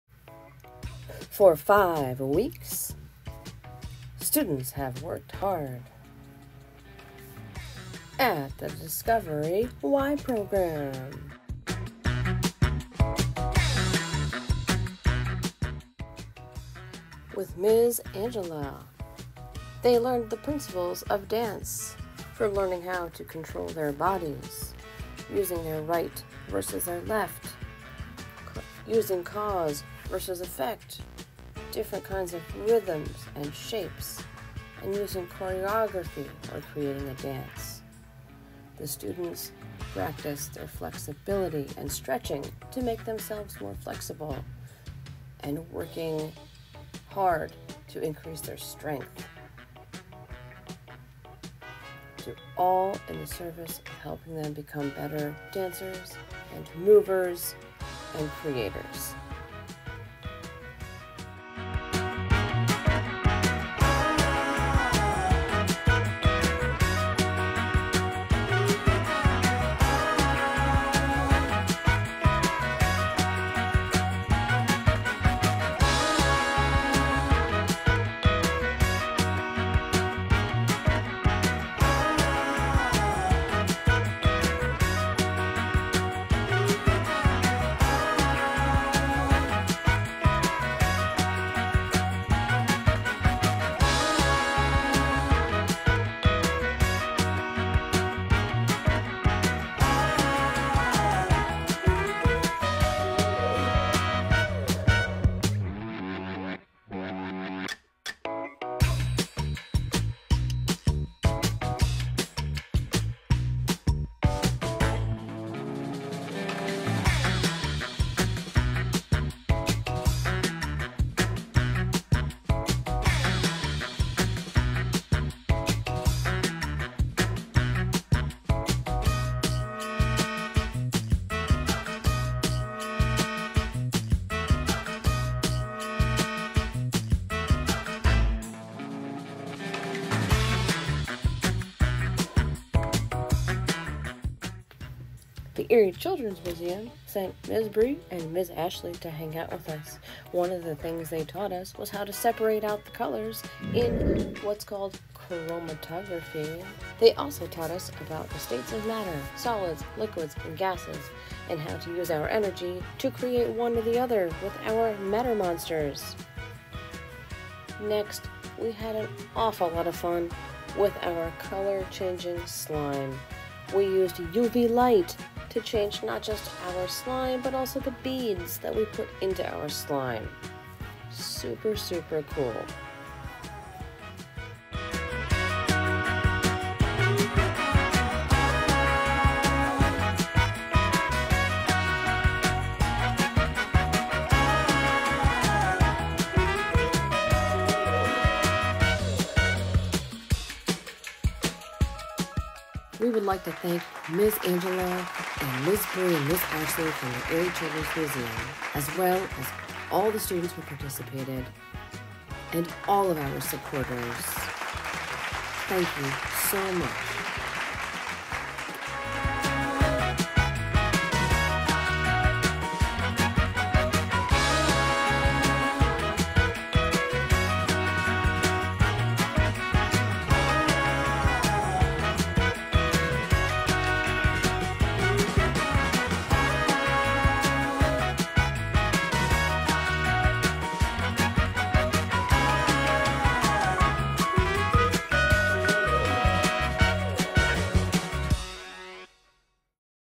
DiscoverY Session 1
For 5 weeks first graders from Pfeiffer-Burleigh Elementary school worked with teaching artists, a dancer and clay artists, as well as their STEAM teacher. They discovered fun and a whole lot of learning!